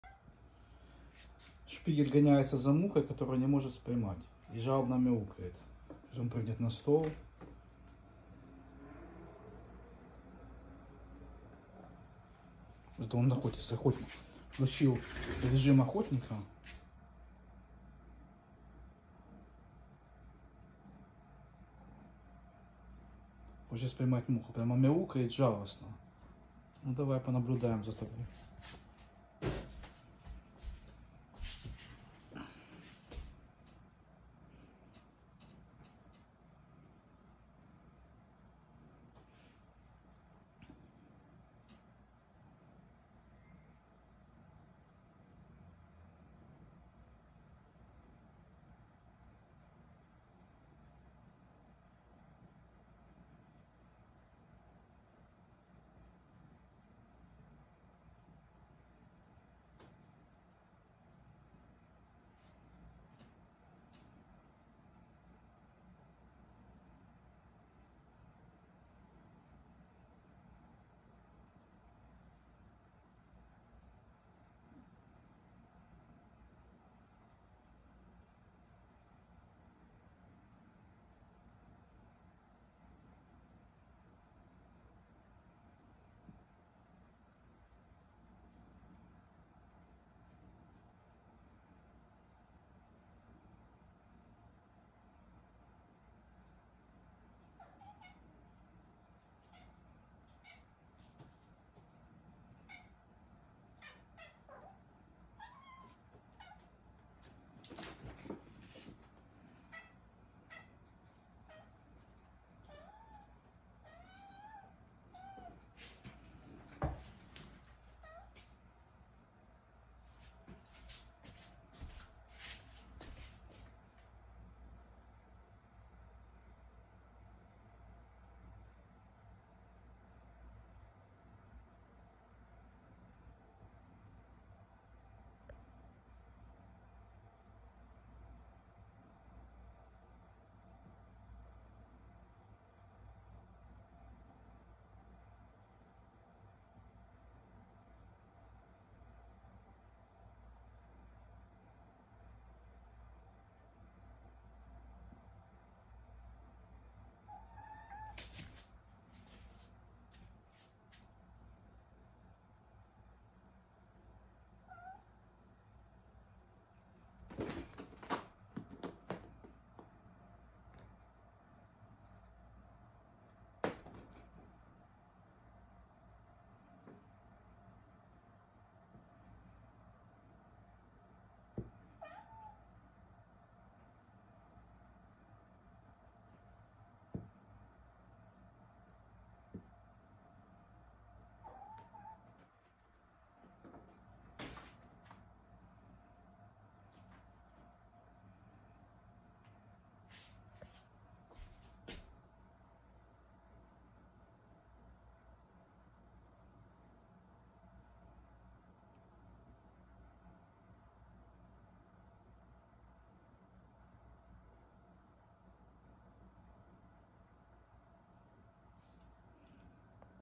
Котёнок 🐈‍⬛ Шпигель охотится на комара и мяукает
Котёнок 🐱 включил режим охотника

Рашисты, если вы считаете добром то, что делает Россия в Украине, то пусть это добро вернётся к вам стократно!!!